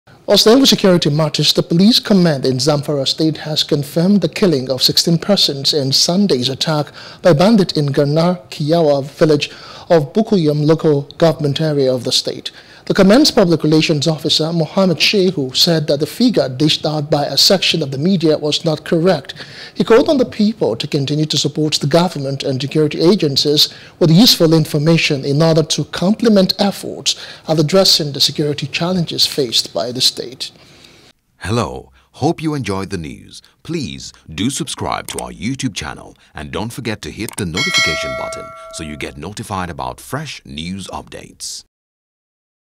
Zamfara Attack: Bandits Killed 16 Persons, Not 24 - Police | NEWS
The Police Command in Zamfara has confirmed the killing of 16 persons in Sunday’s attack by bandits in Ganar Kiyawa village of Bukkuyum Local Government Area of the state.